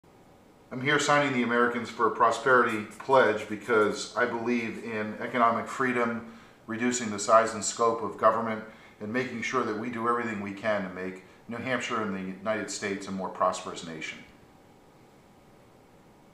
Frank Guinta Signs Americans For Prosperity Pledge
PROMISES TAX AND SPENDING CUTS, CONSTITUTIONALISM